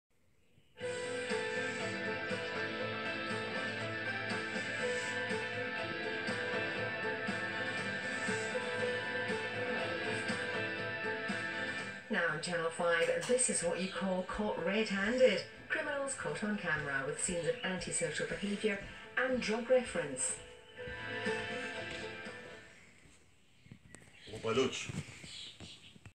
Channel 5 Garnier Ultimate Blends Ident 2015.
This Features People Using Garnier Ultimate Blends Shampoo. Then Back To 5 Stage To The Channel 5 Logo.